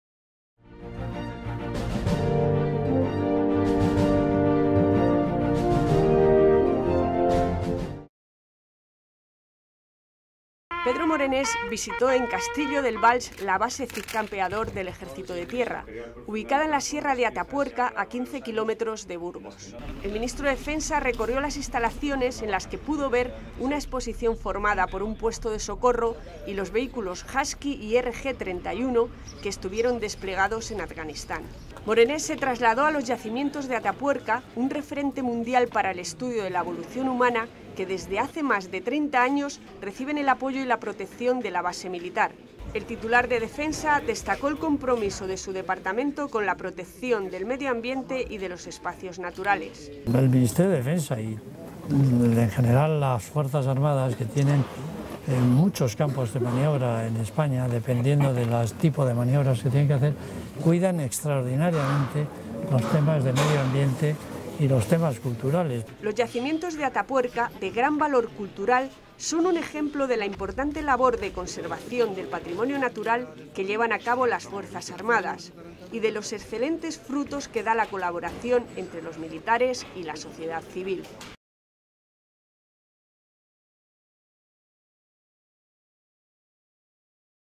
Morenes visita la base Cid Campeador y Atapuerca
Pedro Morenés visitó en Castrillo del Val la base 'Cid Campeador' del Ejército de Tierra, ubicada en la Sierra de Atapuerca, a 15 kilómetros de Burgos.
El ministro de Defensa, acompañado por el jefe del Estado Mayor del Ejército, recorrió las instalaciones en las que pudo ver una exposición formada por un puesto de socorro y los vehículos Husky y RG-31 que estuvieron desplegados en Afganistán.
Morenés se trasladó a los yacimientos de Atapuerca, un referente mundial para el estudio de la evolución humana que, desde hace más de 30 años, reciben el apoyo y la protección de la base militar.
En la Trinchera del Ferrocarril, el ministro recorrió las excavaciones de la Sima del Elefante, la Galería y Gran Dolina acompañado por los codirectores del proyecto Atapuerca que le dieron amplia información sobre los trabajos que se están realizando.
El titular de Defensa destacó el compromiso de su Departamento con la protección del Medio Ambiente y de los espacios naturales.
Los yacimientos de Atapuerca, de gran valor cultural, son un ejemplo de la importante labor de conservación del patrimonio natural que llevan a cabo las Fuerzas Armadas y de los excelentes frutos que da la colaboración entre los militares y la sociedad civil.